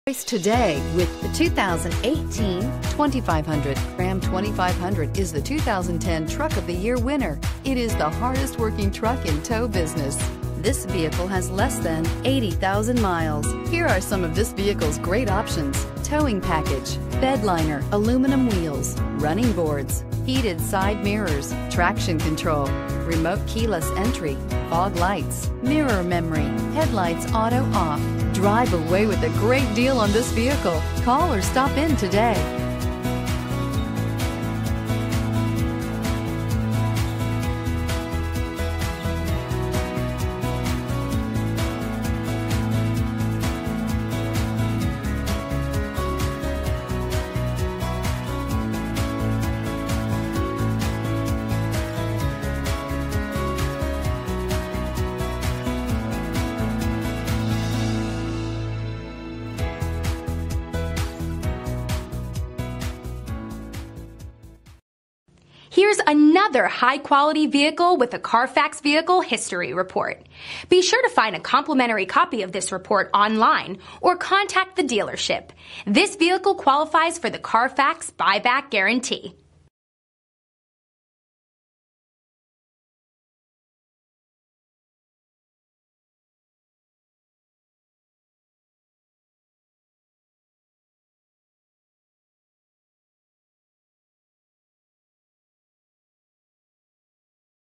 2018 Ram 2500 Gainesville GA G239067A
2018 Ram 2500 Power Wagon

Must print ad and present ad at time of sale to receive quoted price.brbrSunroof, Heated Seats, 4x4, Leather, Clean Carfax.brbrClean CARFAX.brbrFor over 41 years our motto has been welcome to the family Call today for an appointment.